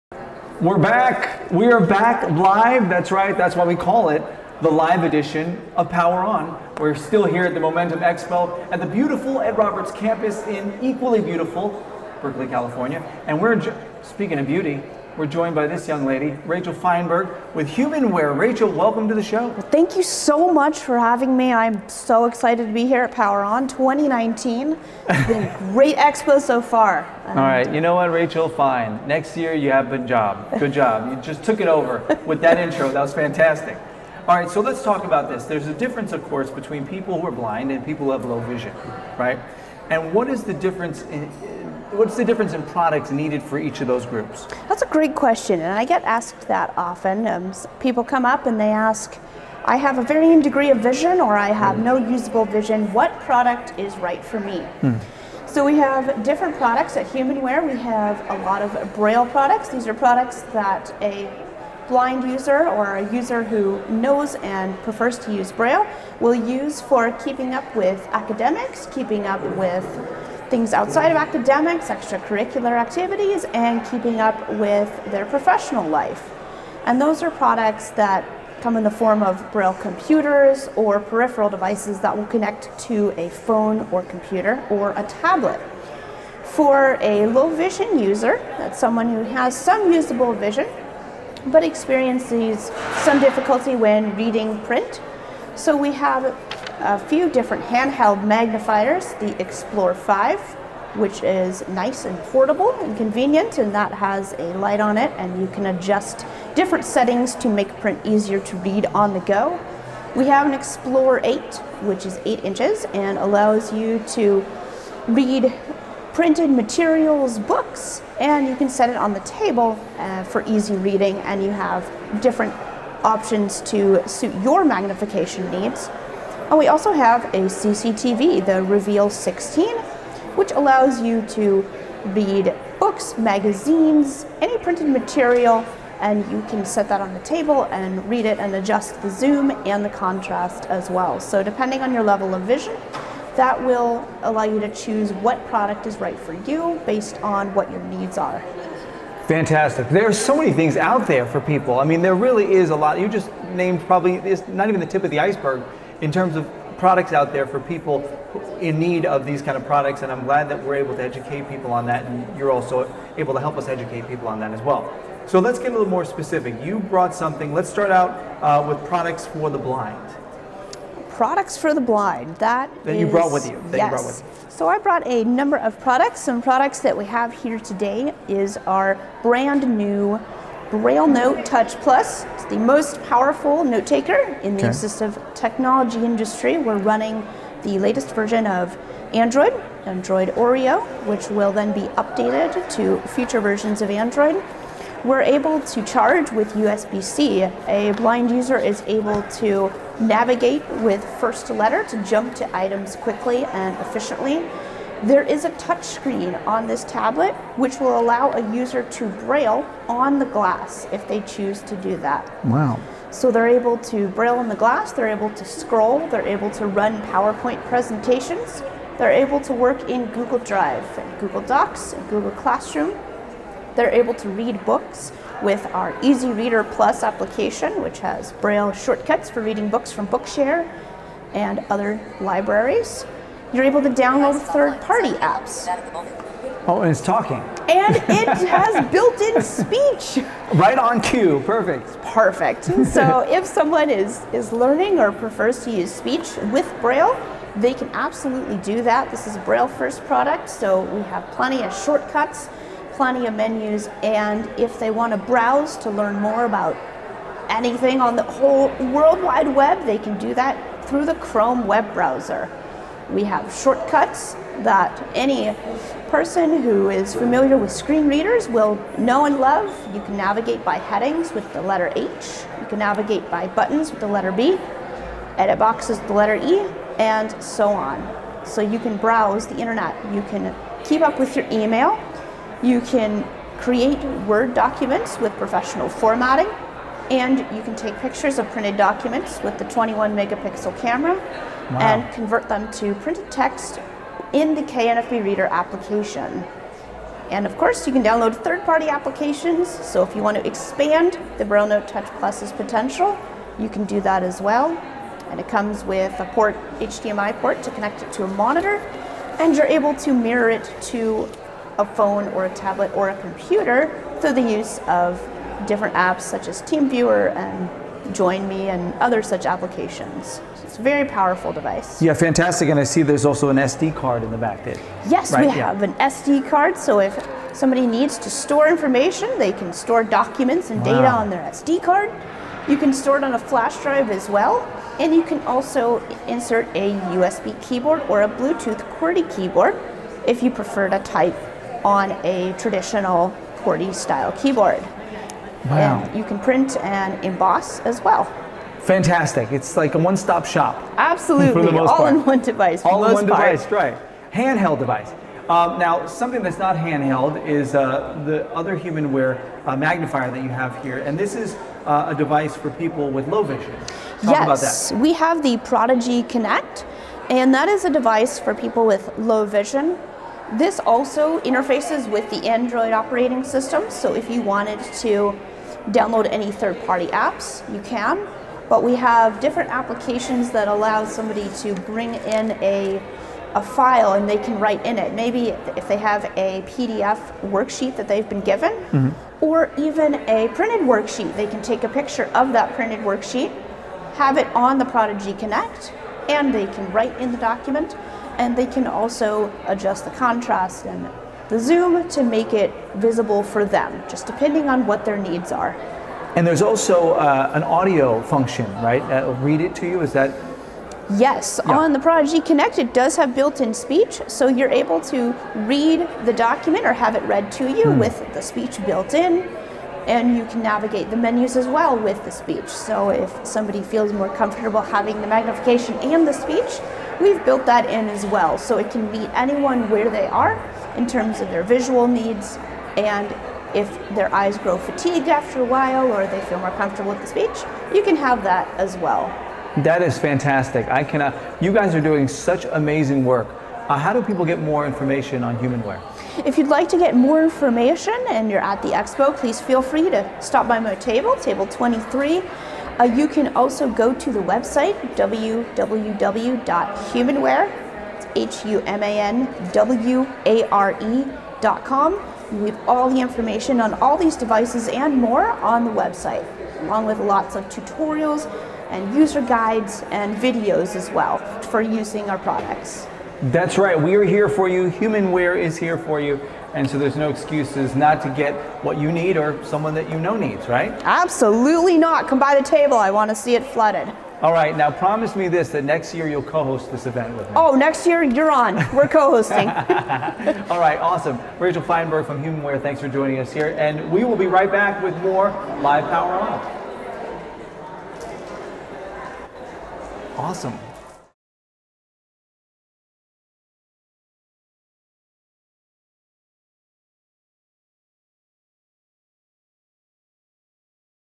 Humanware's Rachel Feinberg Discusses BrailleNote Touch, Prodigi, and More!
Humanware's Product Specialist Rachel Feinberg stopped by our live stream at last month's Momentum Expo to talk about the latest blindness and low vision products from Humanware, the global leader in assistive technology for people who are blind or have low vision.

HumanWare offers a wide range of innovative products, including the BrailleNote Touch, first Google certified braille tablet, the iOS compatible Brailliant braille displays, Victor Reader®, the world's leading family of digital audiobook players, and the unique Connect 12.